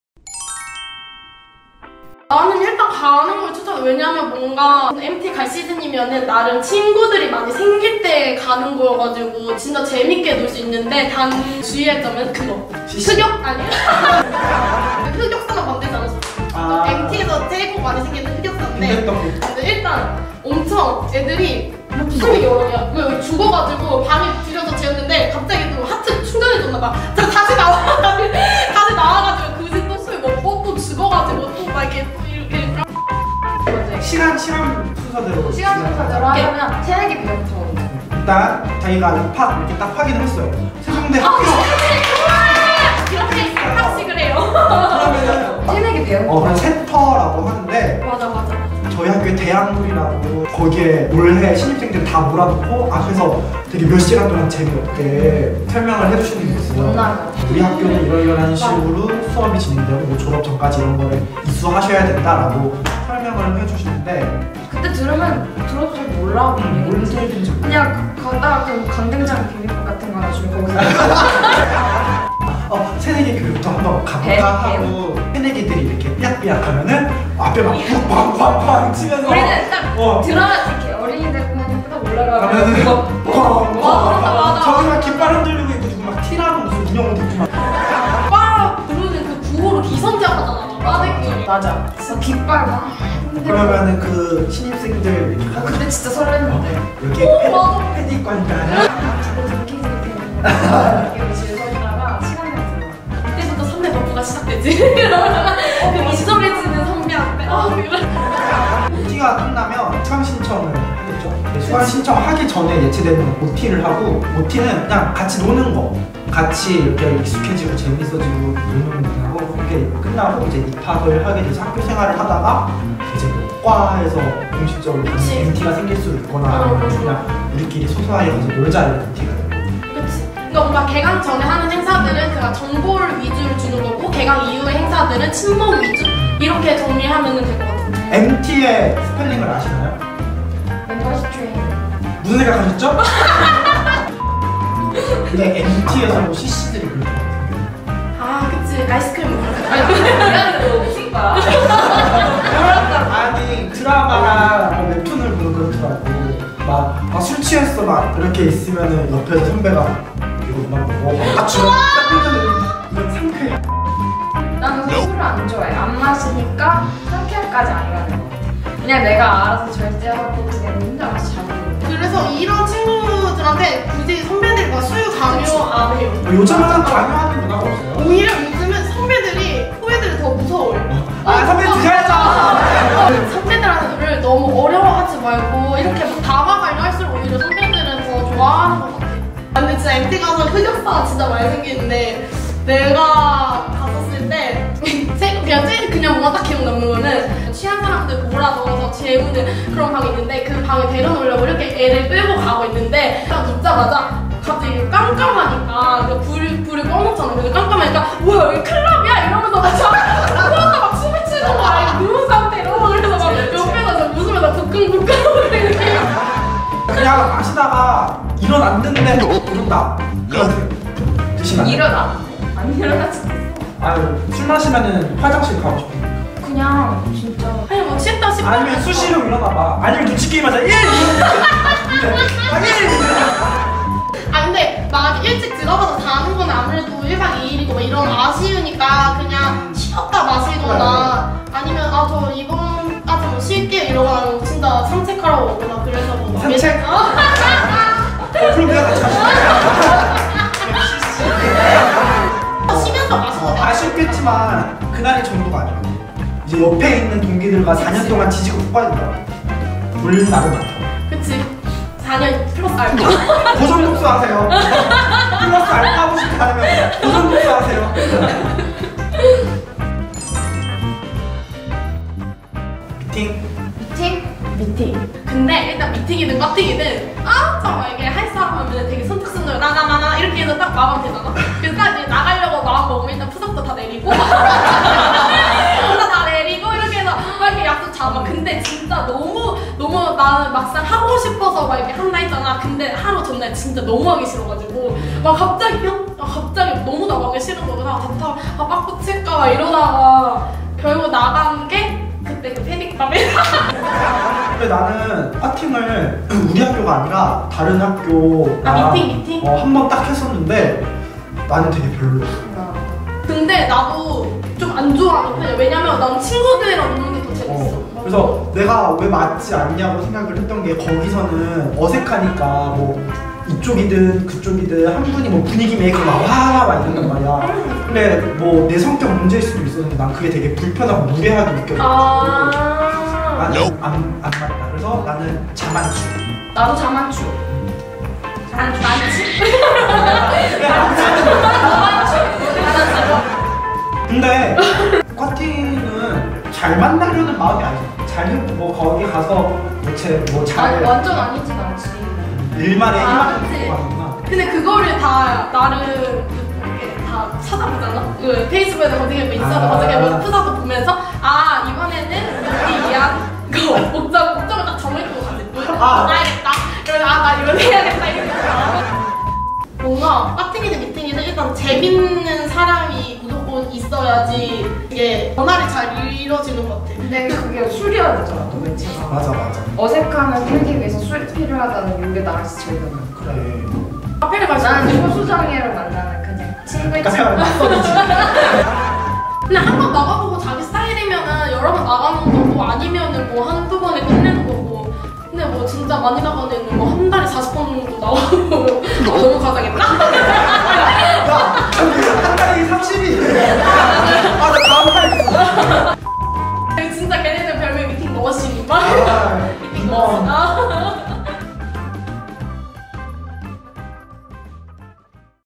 [세종대학교/Sejong University]MT?미팅? 해 말아?! 새내기를 위한 재학생들의 현실 조언!!
안녕하세요~세온이에요:)

대학에 입학한 새내기들의 가장 큰 고민!
MT를 가야할까요?
미팅을 나가야할까요?
대학생활 n년차 재학생들의 현실조언!!
궁금하다면 영상 시청하러 GO GO~

세종대의 더 많은 소식을 듣고 싶다면?